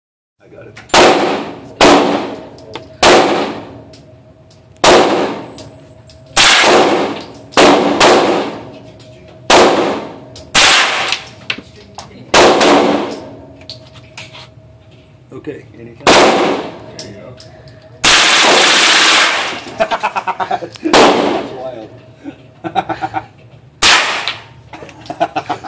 MP5 Suppressed
My dad shooting his first machine gun!